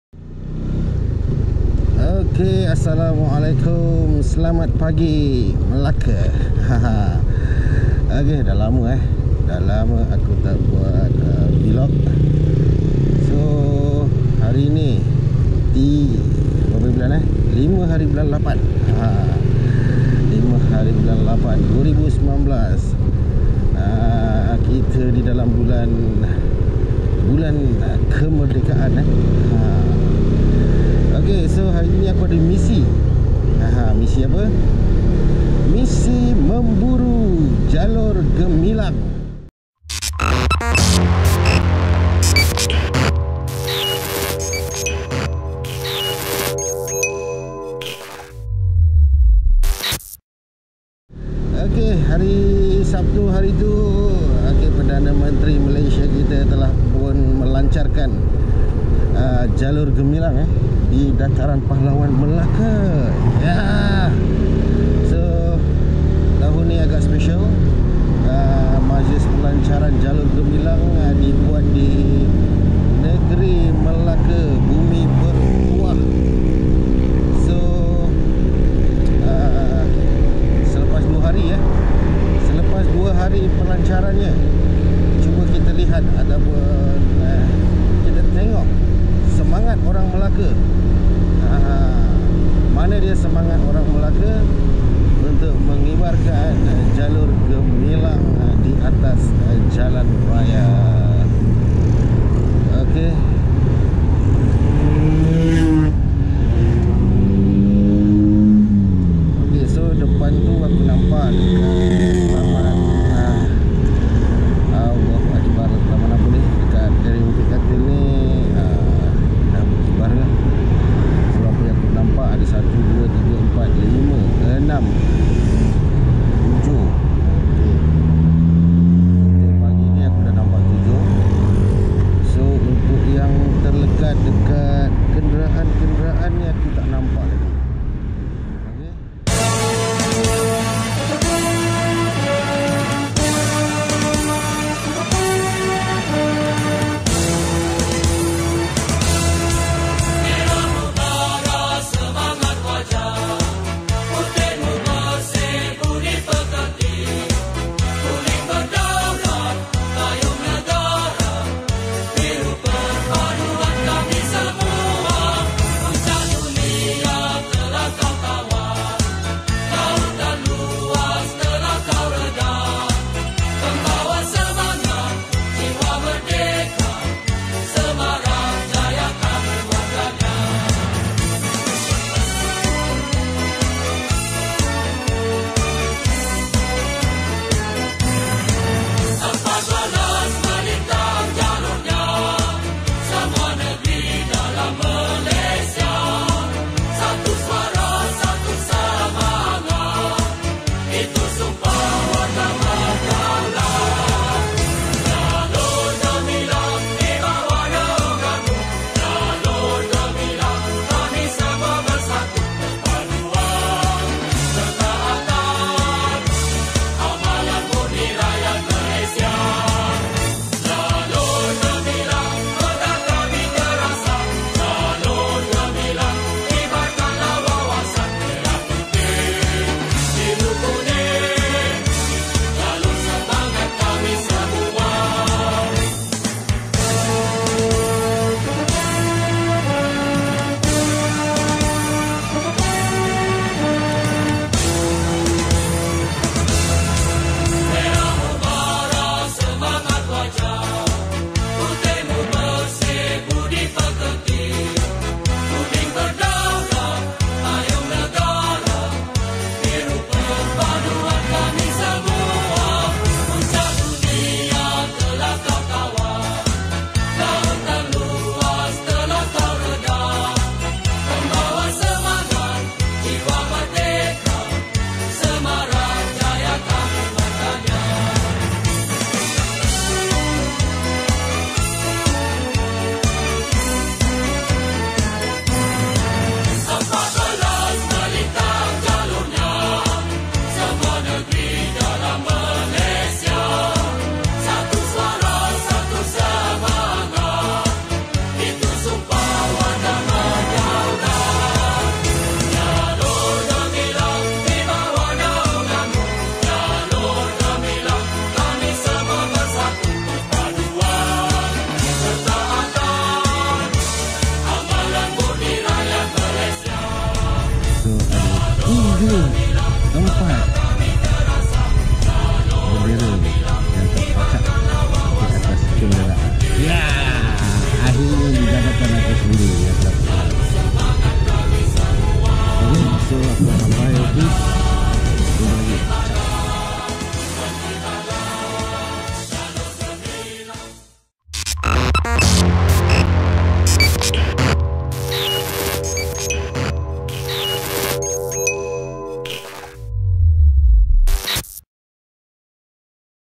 Misi Memburu Jalur Gemilang 2019 (Early Satge) by Mr.RMK Channel
2 hari selepas Pelancaran Kibar Jalur Gemilang oleh YAB Perdana Menteri Malaysia, Tun Dr.Mahathir di Dataran Pahlawan Melaka bersama YAB Ketua Menteri Melaka, Tn. Hj. Adly bin Zahari. Mr. RMK memburu semangat dan kesedaran orang ramai terhadap "Jalur Gemilang" Mr. RMK akan cuba memburu dalam 3 peringkat dan Vlog ini adalah Stage yang pertama.